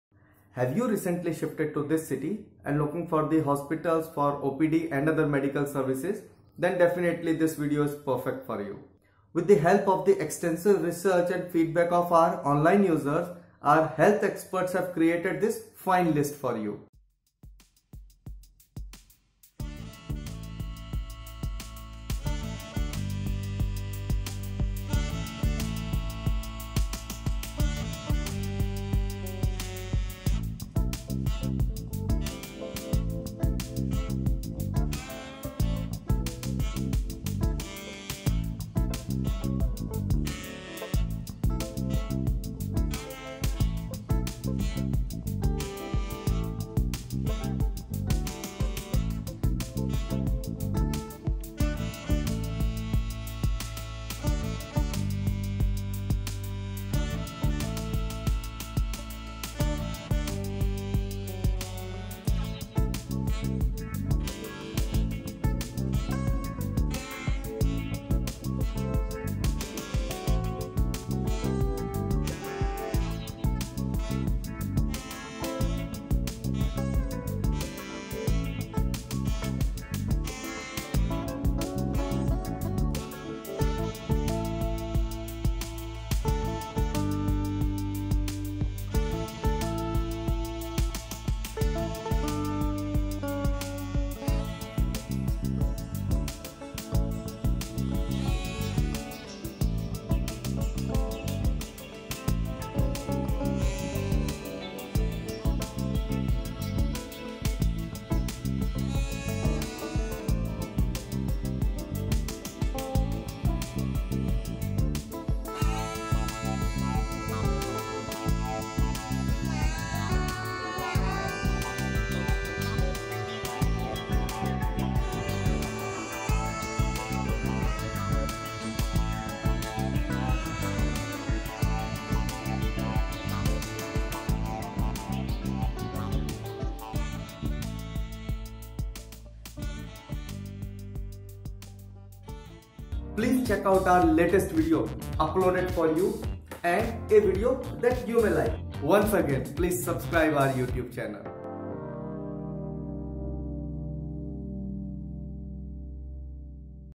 Best Hospitals in Mariara, Venezuela
Must Watch…. Best Hospitals and Clinics in Mariara

List of Best Hospital and Clinics in Mariara:

Simón Bolívar Hospital
Carretera Nacional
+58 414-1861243

Central Hospital of Maracay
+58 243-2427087

Hospital Militar Dr Elbano Paredes Vivas

Hospital de Clinicas Las Delicias
callejón Bocono, Avenida Las Delicias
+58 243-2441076

Hospital Los Samanes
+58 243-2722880

Hospital Jose Maria Vargas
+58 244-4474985

Sociedad Anticancerosa Maracay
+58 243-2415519

Hospital J. M. Carabaño Tosta I.V.S.S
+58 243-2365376

Clínica El Carmen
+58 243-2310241

Centro Médico de Atención Social CANAOBRE
Prolongación de Perez Almarza, entre Paez y Negro Primero, Maracay. al lado del Banco de Venezuela y Diagonal al Centro Comercial de la Economía Informal, Blvd. Pérez Almarza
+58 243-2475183

Hospital Cnel Elbano Paredes Vivas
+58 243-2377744